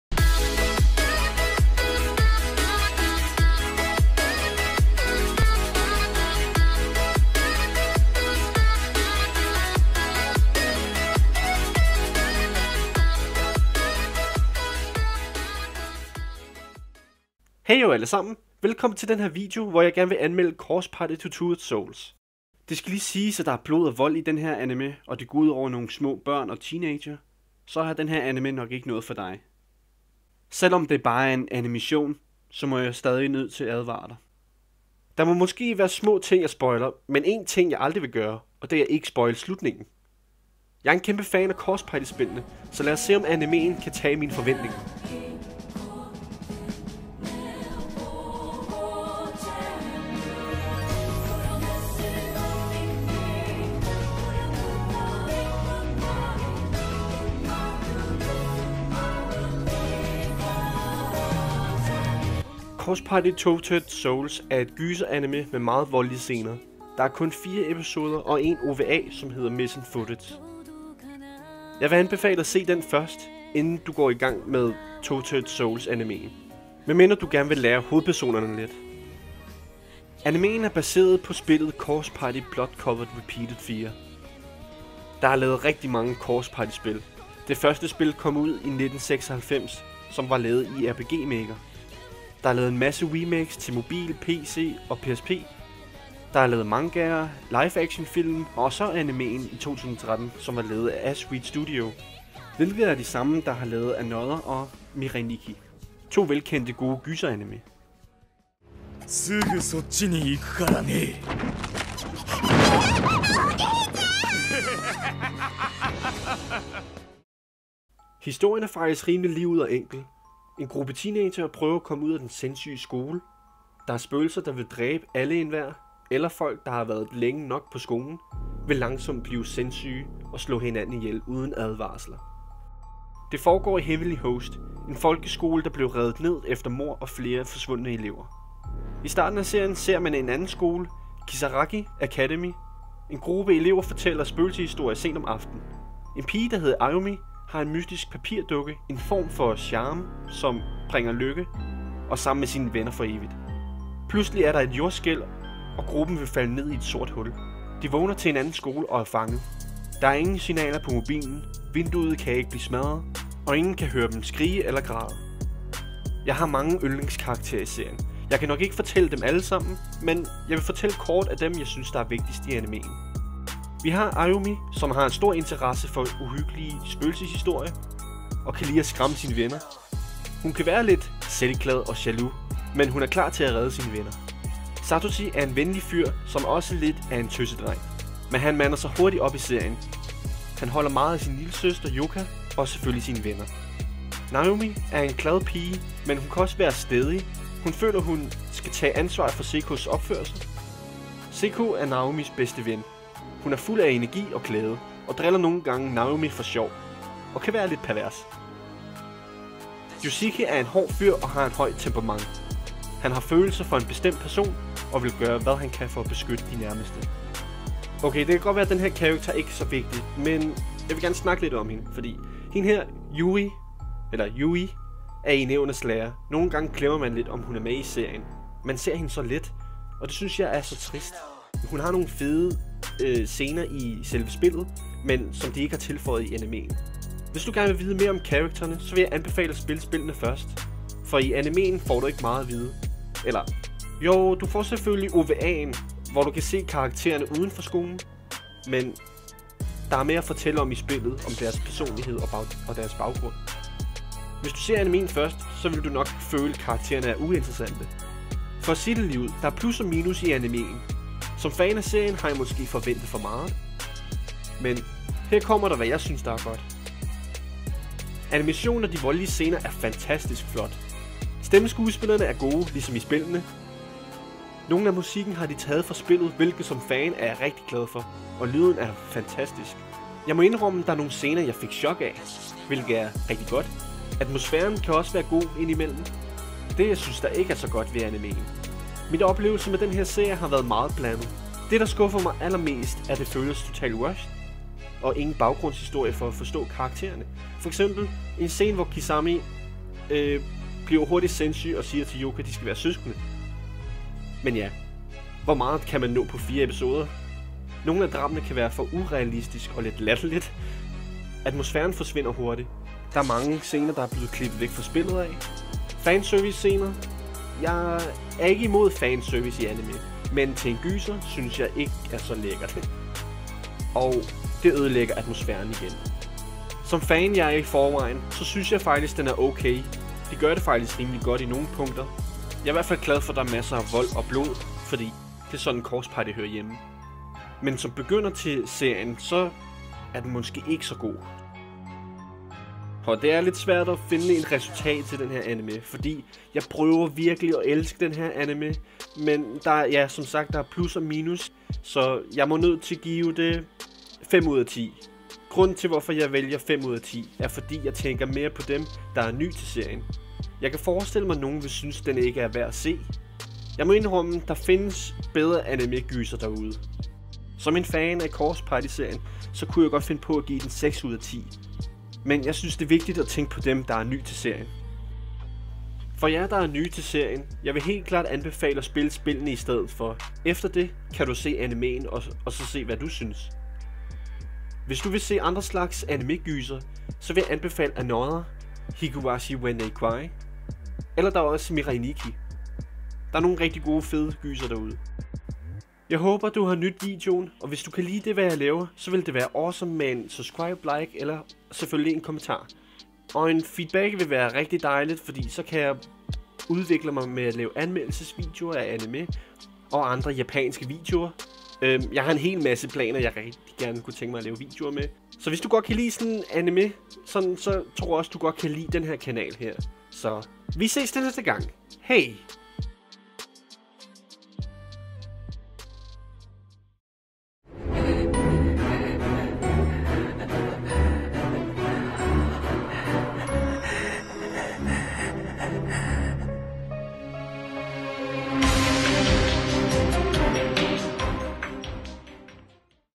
Corpse Party Tortured Souls (Dansk Anmeldelse)
ADVARSEL: Denne video indholder animeret vold og blod. Corpse Party Tortured Souls er R rated 17+

I denne video vil jeg anmelde Corpse party tortured souls. En gyser anime som er baseret på spillene. Jeg elsker corpse party spillene, så lad os se om animeen kan tage i mod mine forventninger...

Hvad synes du om videoen? Skal jeg forsætte med en rating system? jeg vil meget gerne høre din feedback :3 Tak for du ser med!